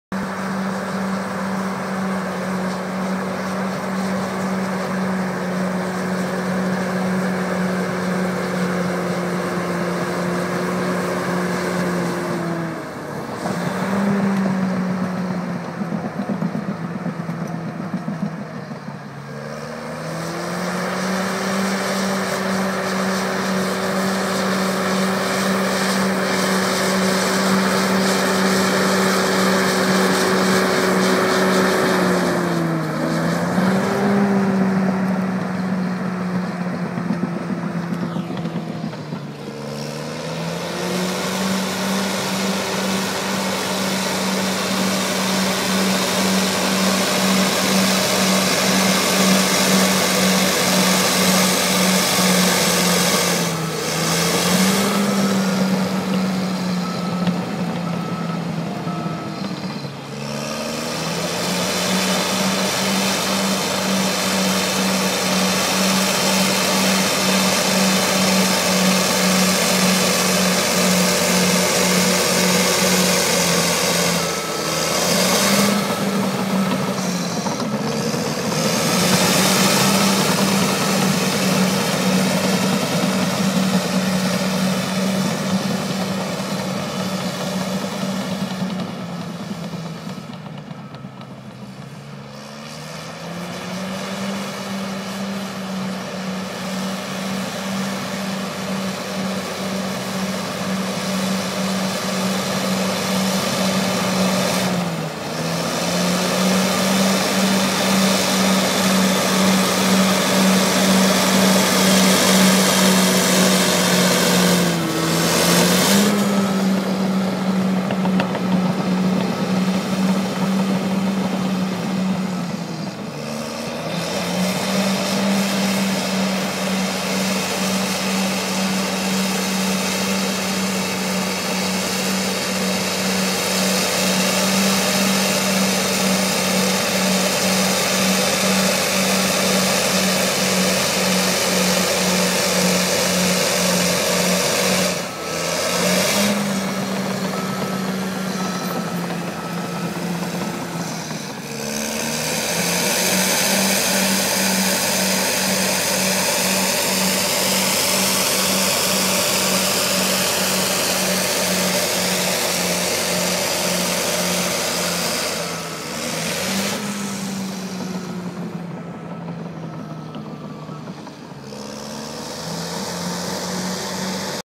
Caterpillar Bulldozer
Caterpillar bulldozer working on the dyke in Zaltbommel